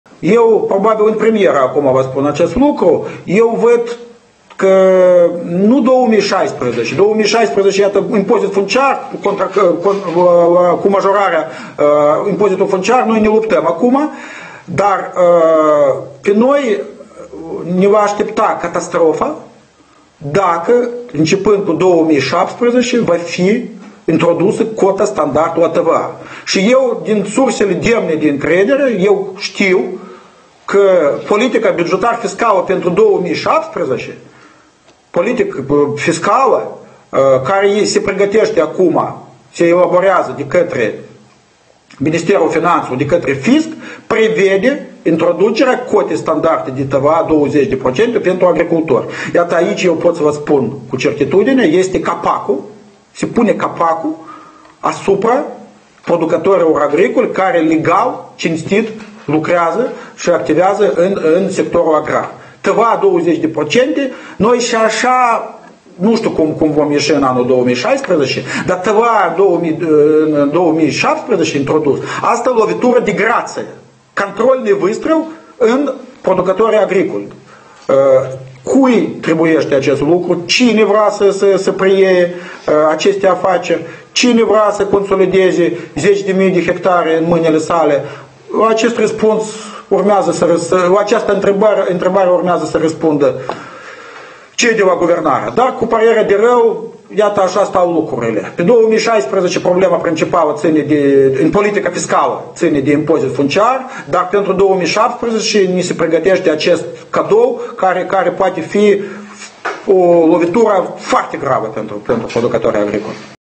Conferință de presă organizată de membrii consiliului administrativ al UniAgroPr...
Alexandr Slusari: Politica bugetar-fiscală pentru anul 2017 prevede taxe mai mari
Titlul evenimentului: Conferință de presă organizată de membrii consiliului administrativ al UniAgroProtect cu tema „Referitor la demisia preşedintelui UniAgroProtect”